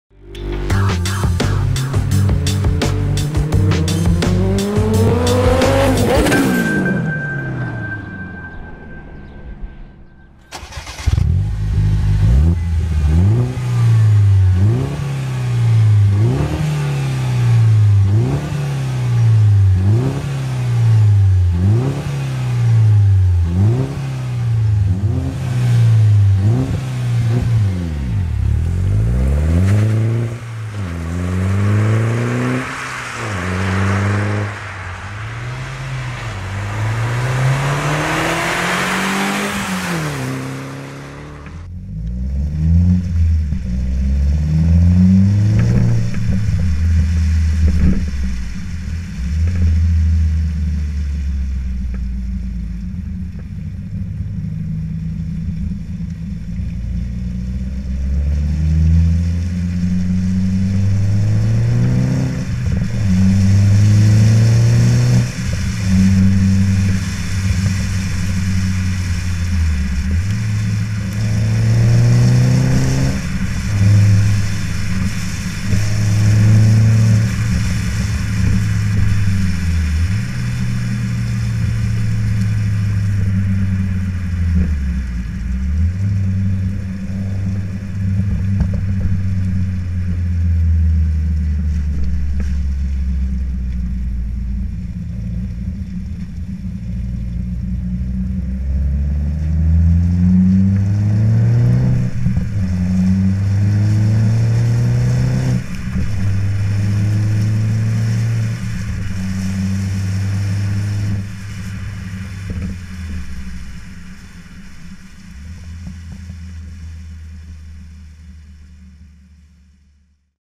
Cobra Sport Mini MK2 R56 Resonated Cat Back Performance Exhaust System
Example of the stainless steel resonated cat back performance exhaust for the Mini MK2 R56 produced by Cobra Sport in Sheffield, UK.

Made in the UK, this exhaust system is manufactured in T304 Grade Stainless Steel, with mandrel bent pipework and Tig welded which allows a smooth flow to maximize power gains. Designed to achieve a sporty sound and our quality is backed up with a lifetime warranty!

Please note: This video was taken on private land with the consent of the vehicle owner.